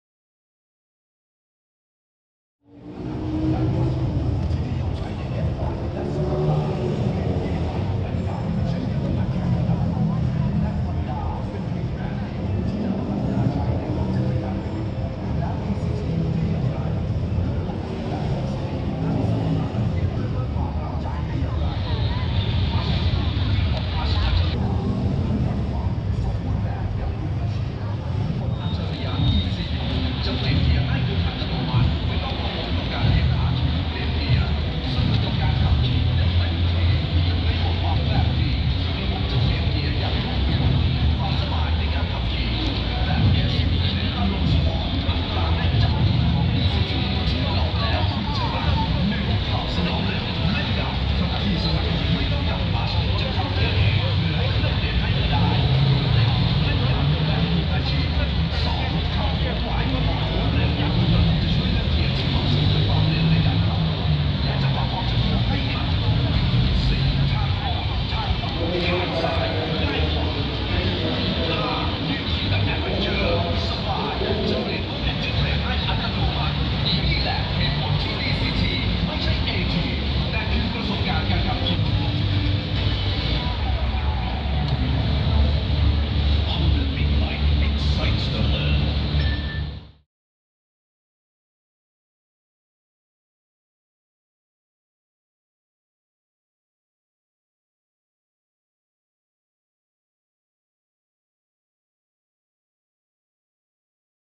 New 2020 Honda ADV150 Bangkok international motor show Thailand
the New 2020 Honda ADV 150 at the Bangkok international motor show Thailand, the All New Forza 350 at my channel and a link at the end of this video, enjoy. oh! lots more from the motorcycle show on the way.. .